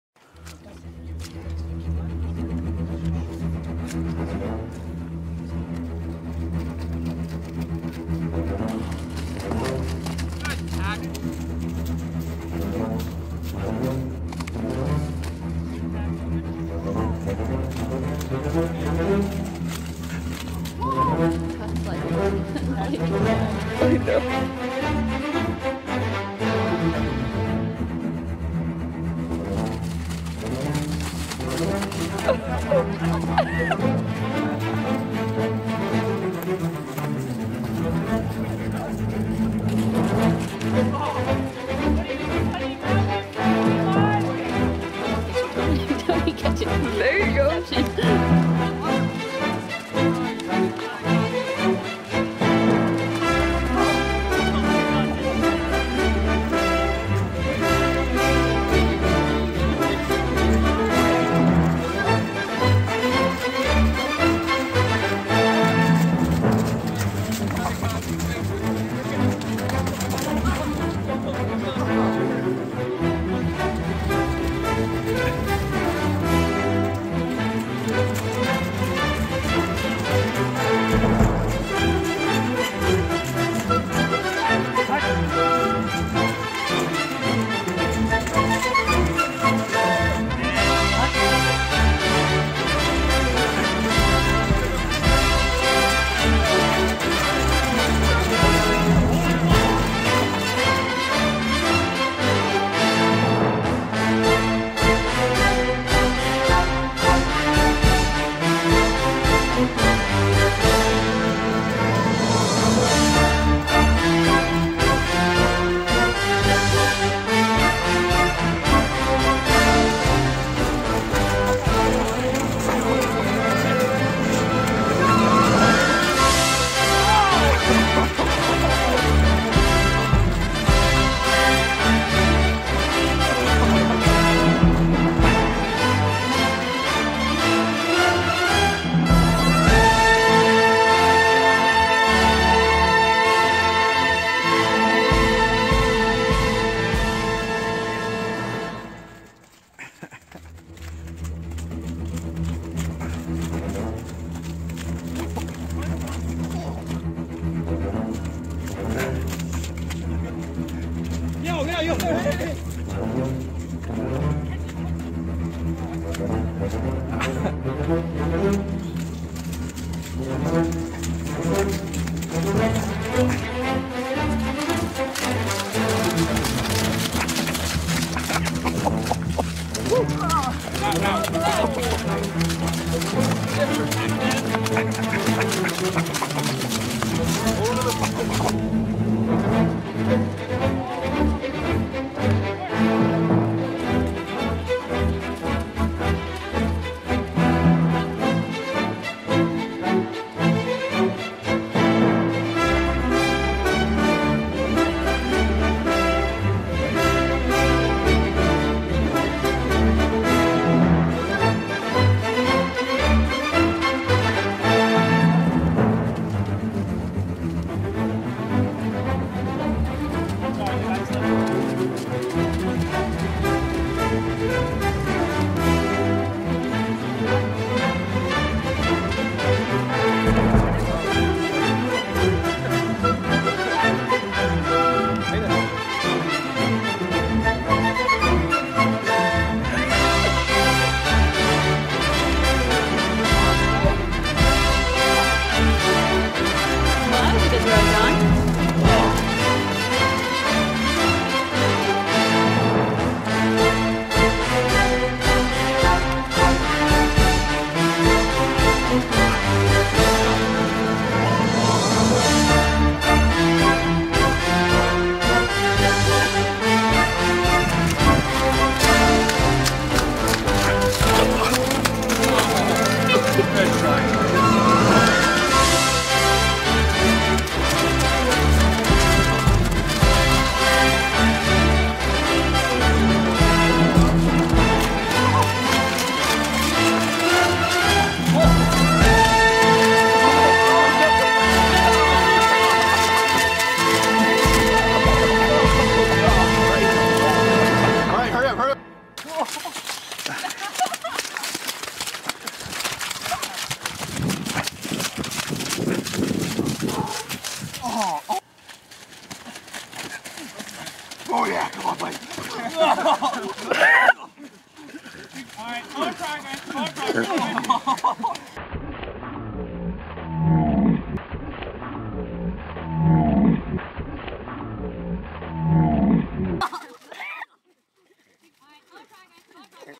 Muddy Backyard Football Game (Dec. 2018)
A messy football game just after Christmas.